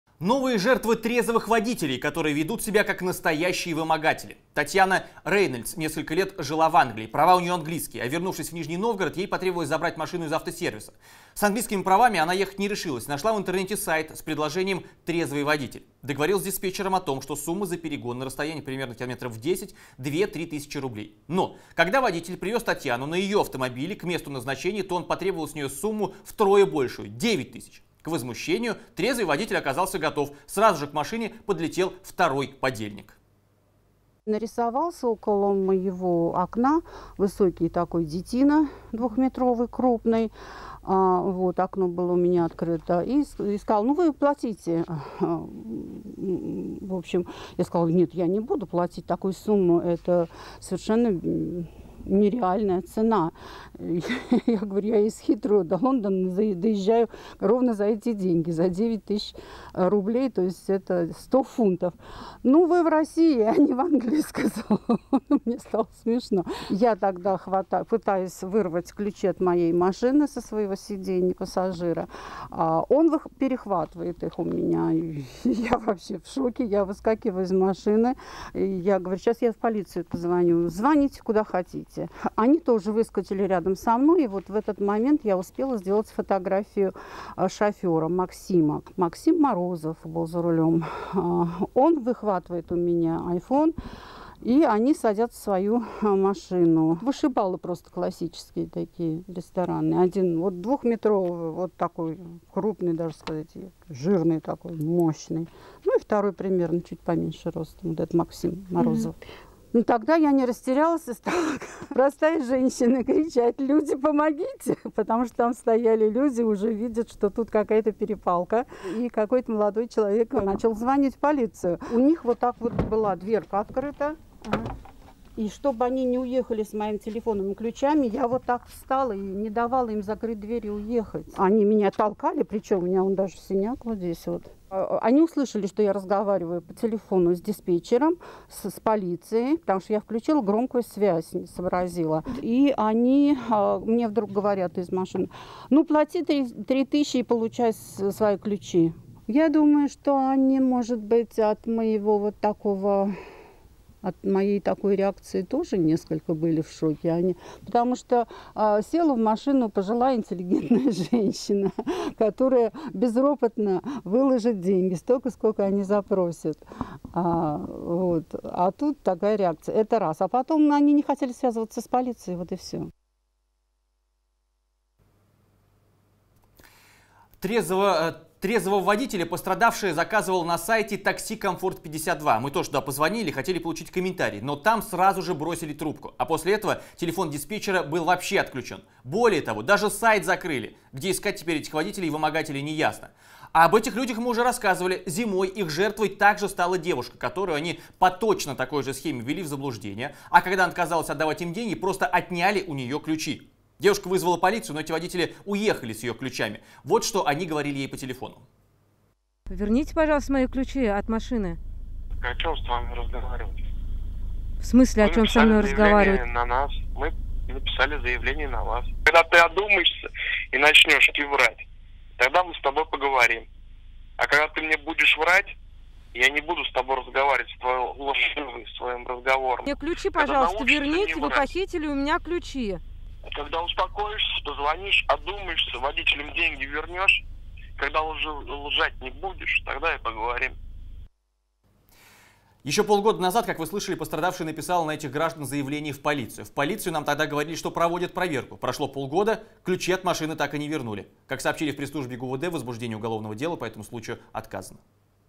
Нижегородка, вернувшаяся из эмиграции, стала жертвой «трезвых водителей»
Новые жертвы «трезвых водителей», которые ведут себя как настоящие вымогатели. Татьяна Рейнольдс несколько лет жила в Англии - права у нее английские, а вернувшись в Нижний Новгород ей потребовалось забрать машину из автосервиса. С английскими правами она ехать не решилась, нашла в интернете сайт с предложением «трезвый водитель». Договорились с диспетчером о том, что сумма за перегон на расстояние примерно в 10 км - составит 2-3 тысячи рублей. Но когда водитель привез Татьяну на ее автомобиле к месту назначения, то он потребовал с нее сумму втрое больше - 9000. К возмущению "трезвый водитель" оказался готов - сразу же к машине подлетел второй подельник.